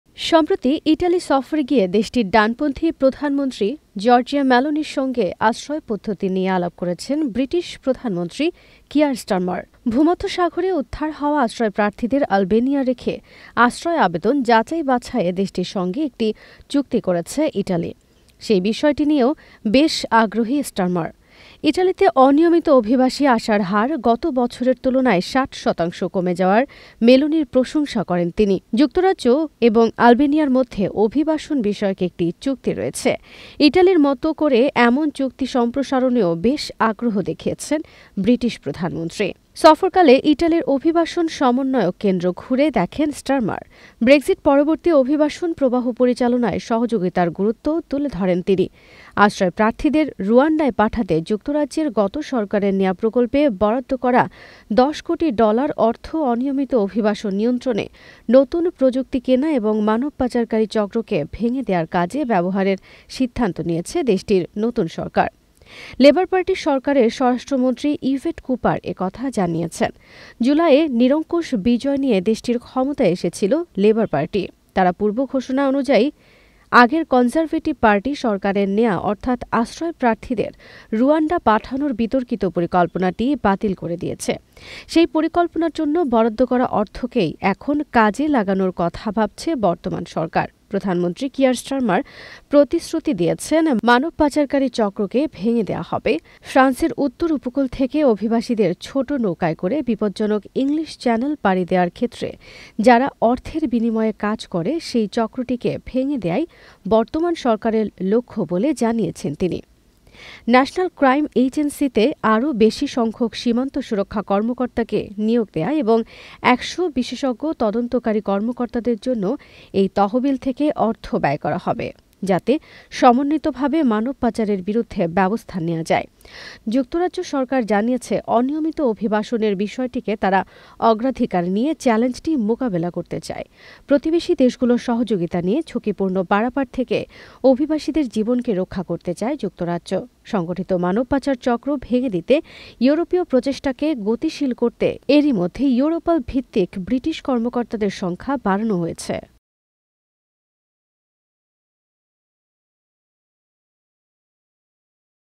অবৈধ অভিবাসী সমস্যা সমাধানে কাজ করবে ইতালি-যুক্তরাজ্য | মানবপাচার রোধে সরঞ্জাম কিনবে ব্রিটেন | BBTV
অবৈধ অভিবাসী সমস্যা সমাধানে একসাথে কাজ করবে ইতালি ও যুক্তরাজ্য | UK | Italy | BBTV

অনিয়মিত অভিবাসী ঠেকাতে ইতালির মতো চুক্তি করতে চায় যুক্তরাজ্য

রুয়ান্ডা প্রকল্পের অর্থ কাজে লাগাতে চায় ব্রিটেন

অবৈধ অভিবাসী সমস্যা সমাধানে একসাথে কাজ করবে ইতালি ও যুক্তরাজ্য | রুয়ান্ডা অর্থ দিয়ে মানবপাচার রোধে সরঞ্জাম কিনবে ব্রিটেন | BBTV

সম্প্রতি ইটালি সফরে গিয়ে দেশটির ডানপন্থি প্রধানমন্ত্রী জর্জা মেলোনির সঙ্গে আশ্রয় পদ্ধতি নিয়ে আলাপ করেছেন ব্রিটিশ প্রধানমন্ত্রী কিয়ের স্টারমার৷ ভূমধ্যসাগরে উদ্ধার হওয়া আশ্রয়প্রার্থীদের আলবেনিয়া রেখে আশ্রয় আবেদন যাচাই-বাছাইয়ে দেশটির সঙ্গে একটি চুক্তি করেছে ইটালি৷ সেই বিষয়টি নিয়েও বেশ আগ্রহী স্টারমার৷

BB tv Bangla media platform. Our mission working for community.